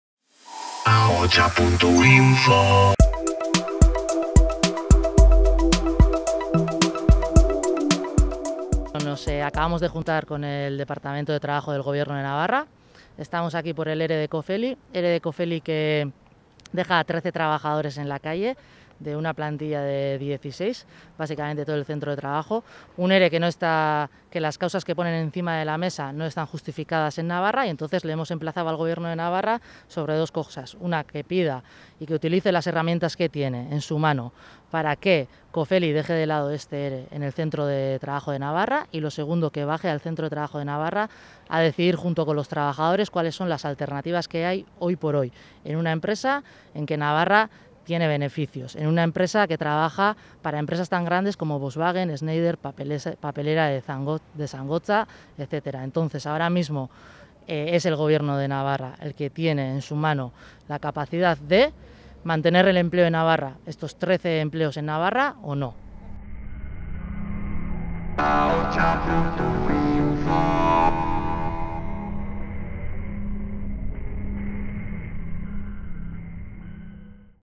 Trabajadores de Cofely piden al Gobierno que intervenga para salvar los puestos de trabajo
Una delegación de trabajadores de la empresa Cofely en Navarra se ha reunido hoy, en Pamplona, con los responsables del Servicio de Trabajo del Gobierno de Navarra y, a la vez que ha trasladado su preocupación por la situación generada, les ha solicitado intervenir en este conflicto laboral. En esta tercera semana de negociación del Expediente de Regulación de Empleo, el sindicato LAB quiere denunciar que la empresa apenas se ha movido de su planteamiento inicial y que, hoy por hoy, se encuentran en riesgo de despido 583 trabajadores en toda la península, 13 de ellos en Navarra (casi toda la plantilla de aquí).